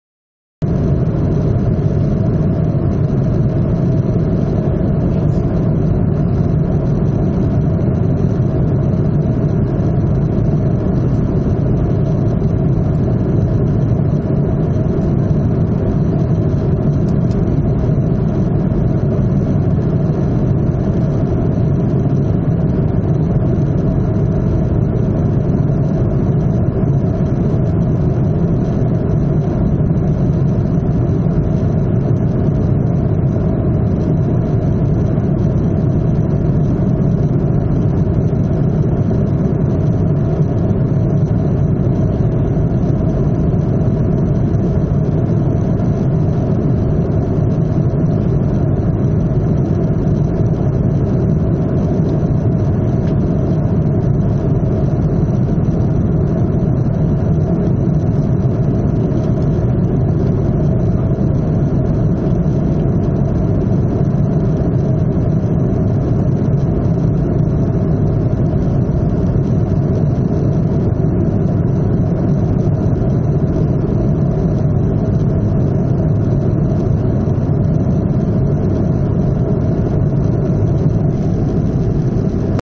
Sunset Di Ketinggian 30000 Kaki [ Travel Vlog Jalan Jalan Di Jepang ]
Karena saya berangkat sore hari, jadi saya mendapatkan kesempatan untuk menikmati pemandangan matahari terbenam dari dalam kabin pesawat. Pemandangan matahari terbenam seperti di video, saya dapatkan di wilayah udara Philipines :D

[ Travel Vlog Jalan Jalan Di Jepang ]